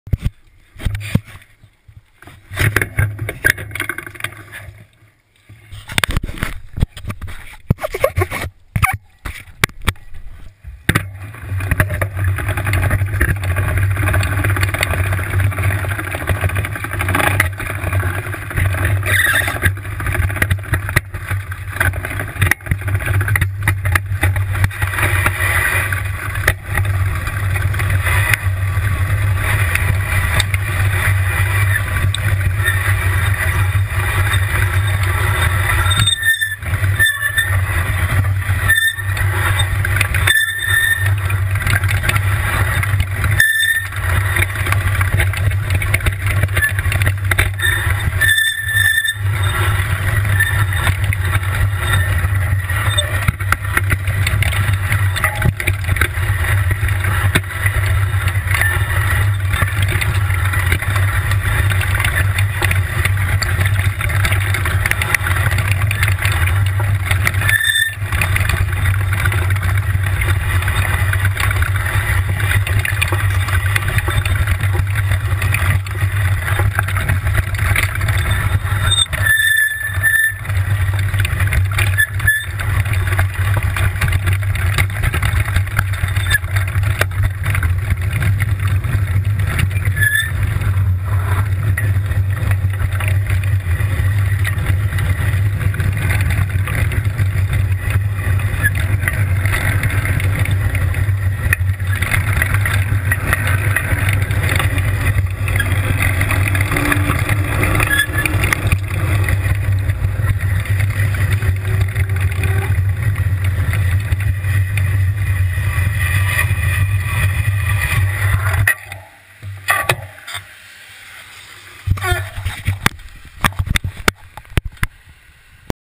Une petite descente boueuse à Cenon
Lors d'une sortie sur la rive droite j'ai tenté une petite descente dans un chemin disons trempé (lol)